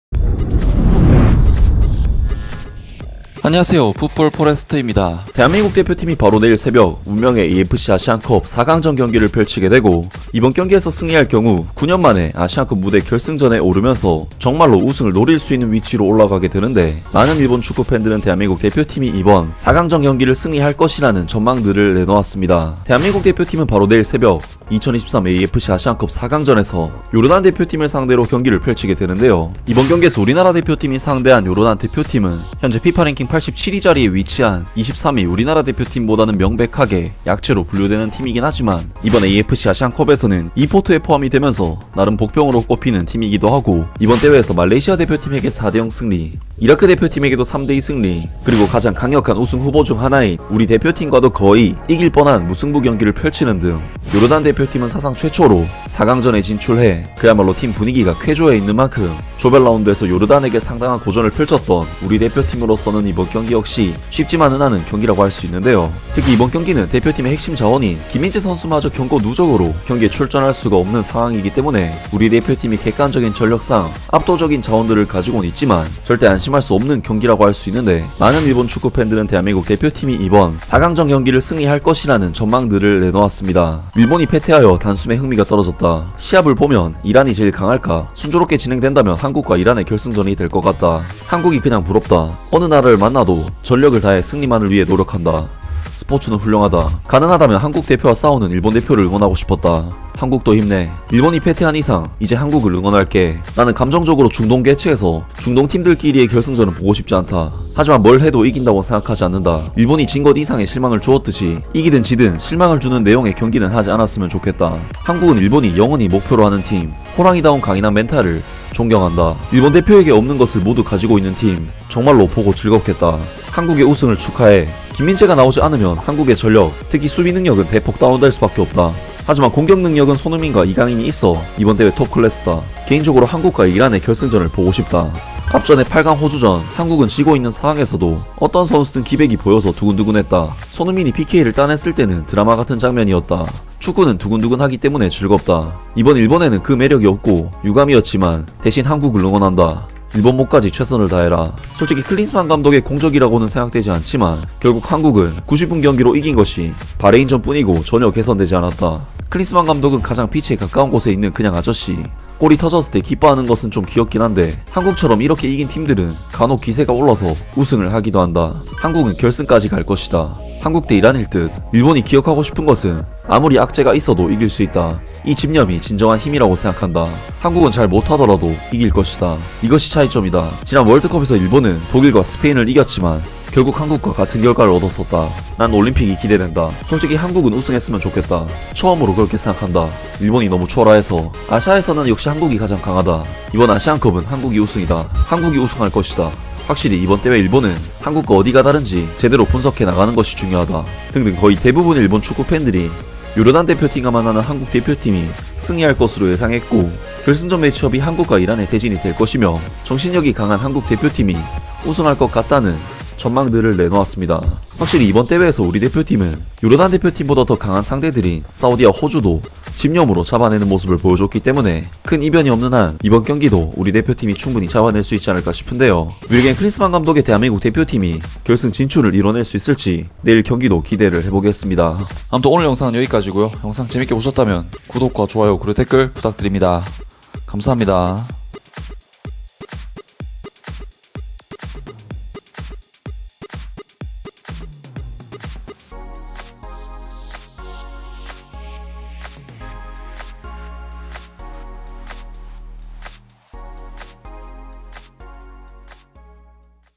"한국이 요르단 이기고 결승 예상" 운명의 4강 요르단전 앞둔 한국 대표팀에게 일본 팬들이 승리를 예상한 이유 "한국 축구는 집념이 강하다." [대한민국 vs 요르단]
구독!